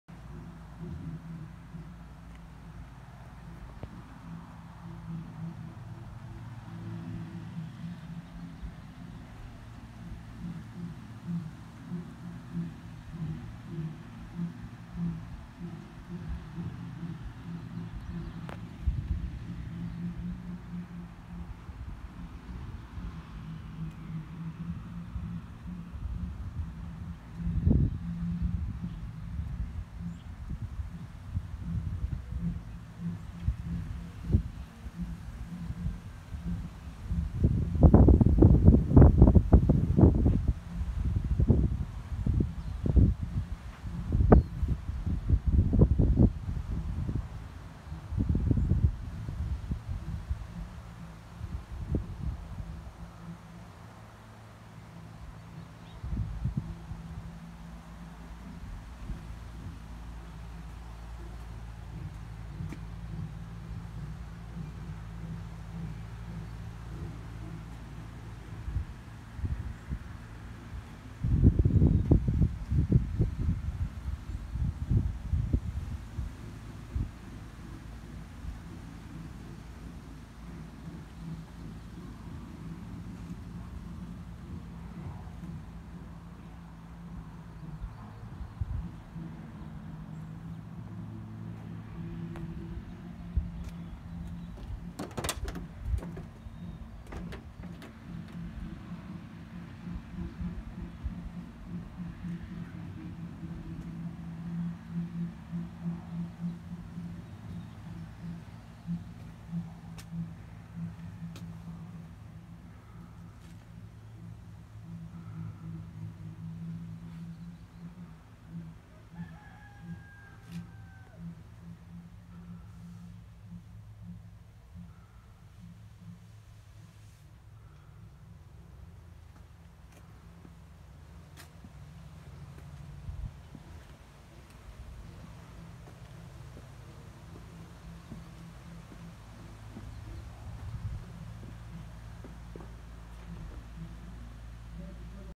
1978-79 Ford F-100 Eng 351
This Truck is in California Make your Offered  it has California plates too